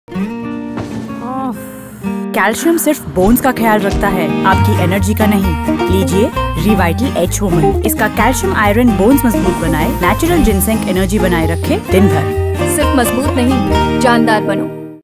Day long energy & stronger bones, with Revital H Women Multivitamin (Hindi) – 15 secs
Revital H Women: Your Ultimate Wellness Companion
Are you ready to unlock a healthier, more vibrant you? Look no further than Revital H Women—a powerhouse multivitamin designed exclusively for women. Key ingredients and benefits:

12 Vitamins and 10 Minerals: Our meticulously crafted formula delivers essential nutrients to support overall health. From Vitamin A to Zinc, we’ve got your well-being covered. Regular consumption of Revital H Women leads to a remarkable 45% improvement in overall health.

Strength and Energy Boost: Need an energy kick? Revital H Women contains three times more zinc, iron, calcium, and ginseng. These elements work together to enhance stamina, mental clarity, and focus. Plus, biotin and selenium contribute to healthy skin, nails, and hair.

30-Day Energy Transformation: Imagine feeling more energetic within just 30 days! Revital H Women improves oxygen-carrying capacity, giving you the vitality you crave. Say goodbye to sluggish days and hello to renewed vigor.

Stress Management Superstars:
       Ginseng: This natural wonder helps manage daily stress, keeping you balanced and centered.
       Iodine: Another stress-busting hero, iodine reduces stress levels by a whopping 50%.

Simple Usage Instructions: Take one tablet daily with a glass of water, milk, or juice. It’s that easy!
Incorporate Revital H Women into your routine and experience the difference.

Experience a notable 28% increase in energy, setting the stage for more vibrant and fulfilling days. And that's not all: within three months, you shall experience a remarkable 45% improvement in your overall health and well-being! Revital H Multivitamins for women - they're all about embracing your power and grace.

Safety Information: Store in a dry place away from heat and sunlight. Read the bottle label carefully before using it. Do not exceed the daily dosage limit, and take the capsule under your doctor’s supervision. Keep away from children.